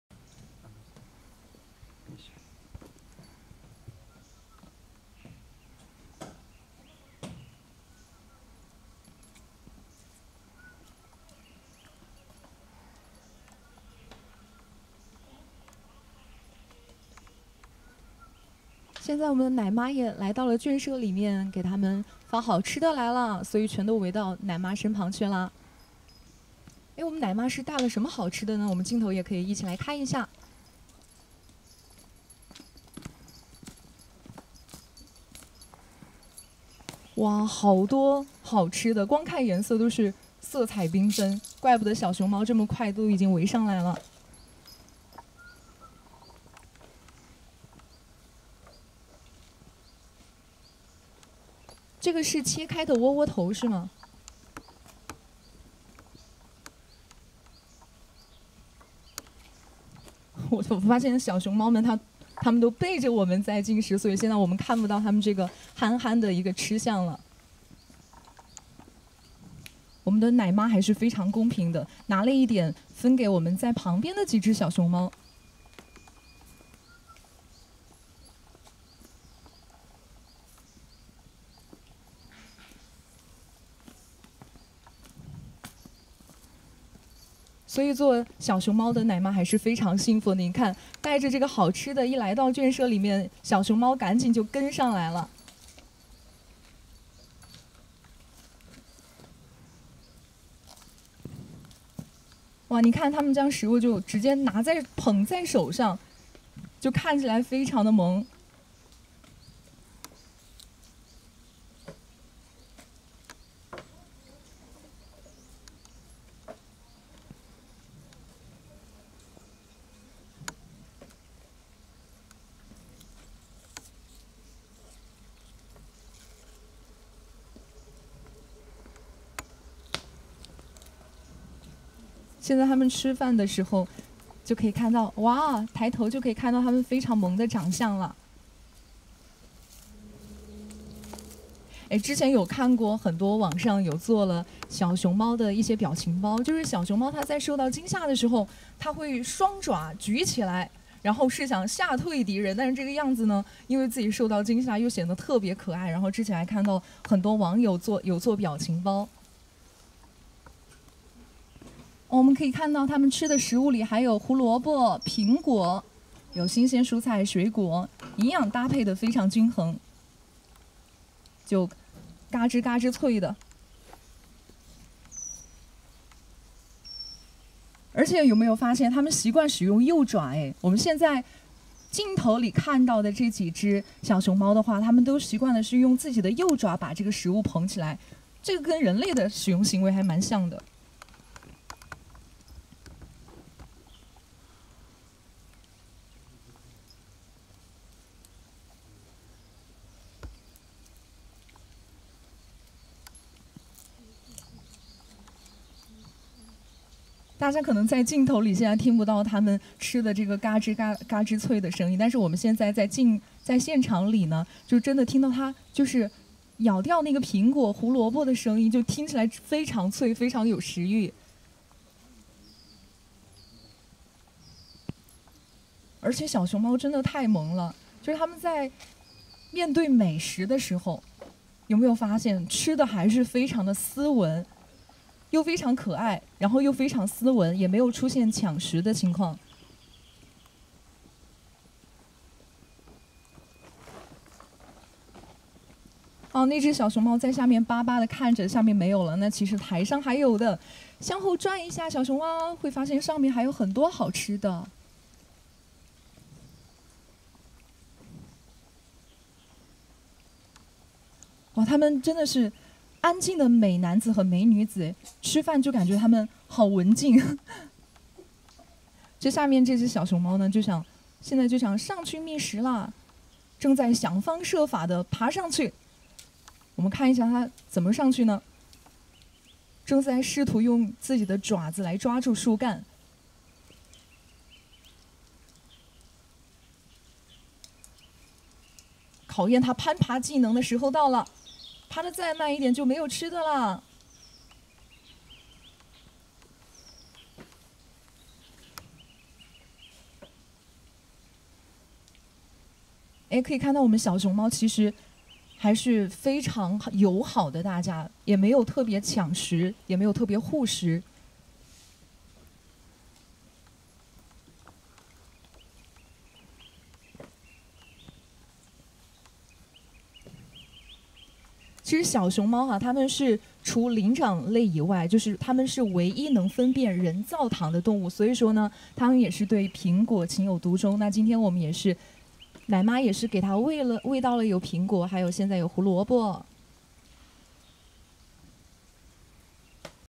Red Pandas’ Dinner Time
It’s red pandas’ dinner time! How is their table manner?